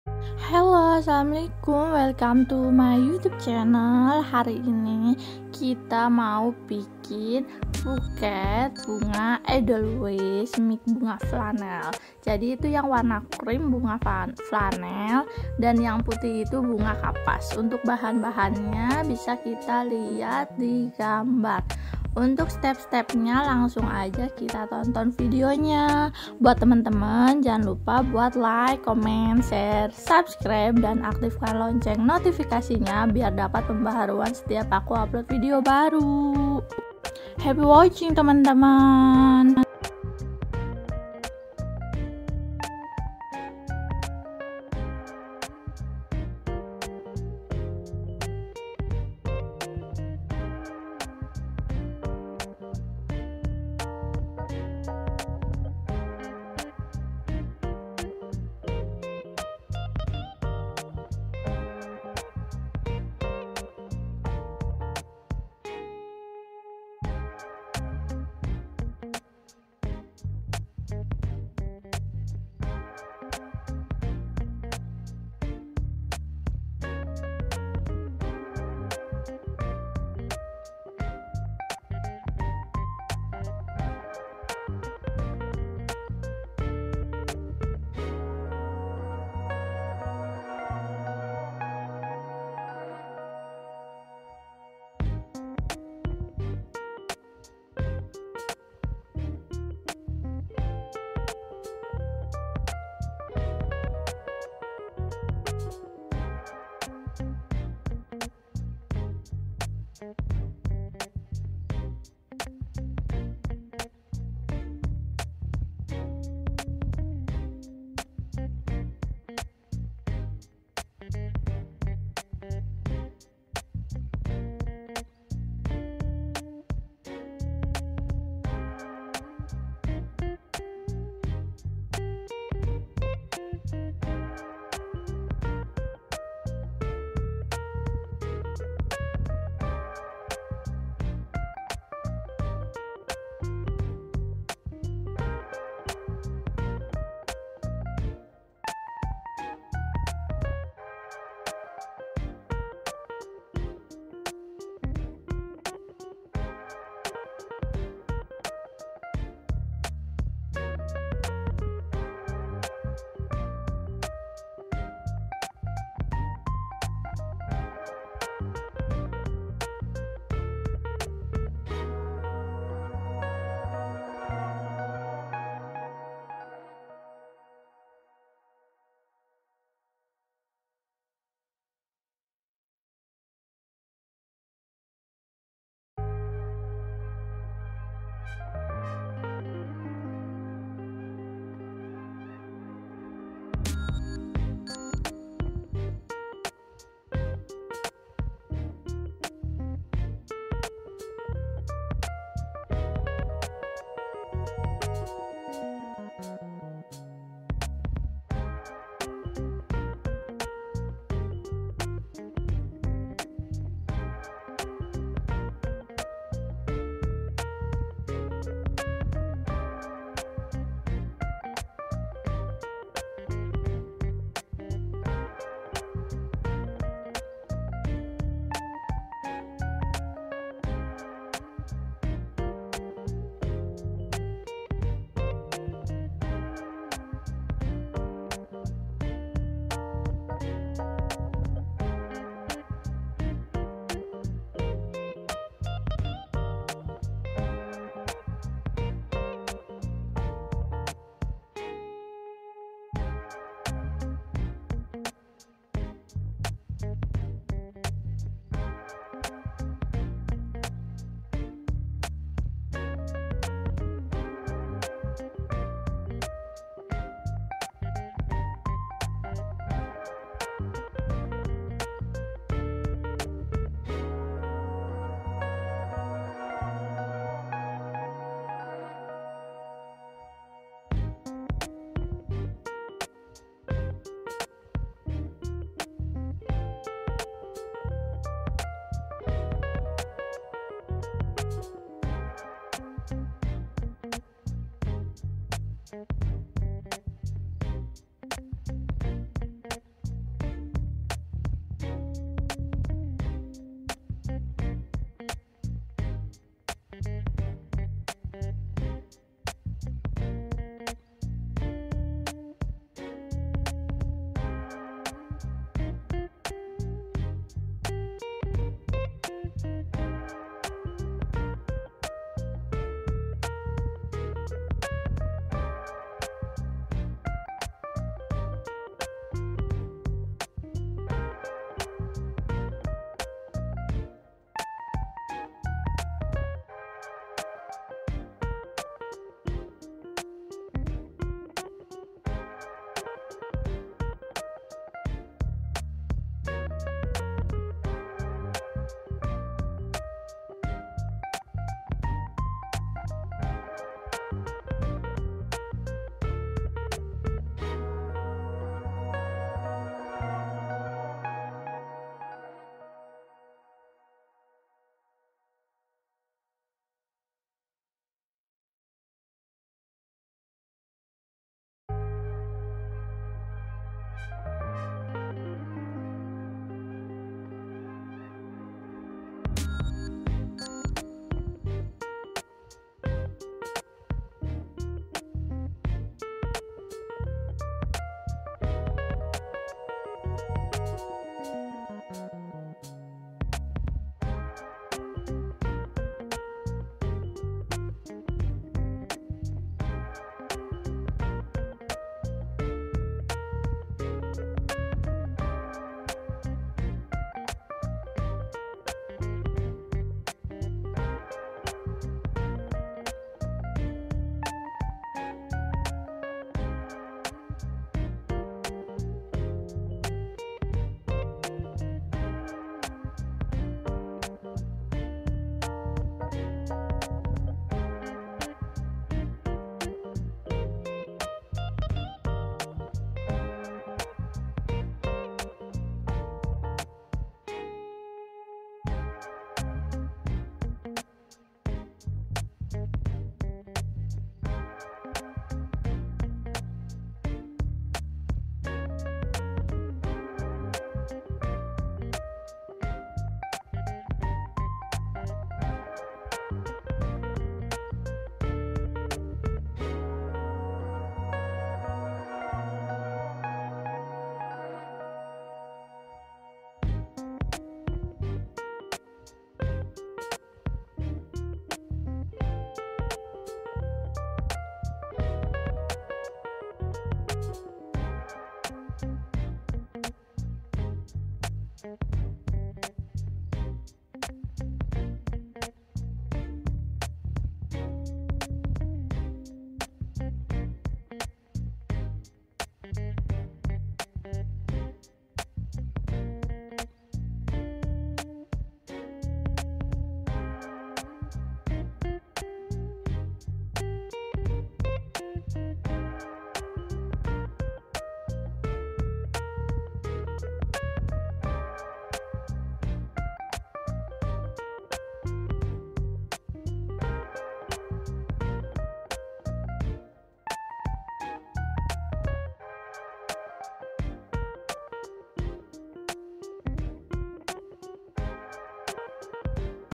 TUTORIAL BUKET BUNGA EDELWEIS CANTIK 💖 || DIY DRY FLOWER BOUQUET
Haii,, kenalin aku isna, di vlog kali ini aku bakal berbagi tentang cara membuat buket bunga edelweis mix bunga flanel

Doain ku rajin upload yah, hiihii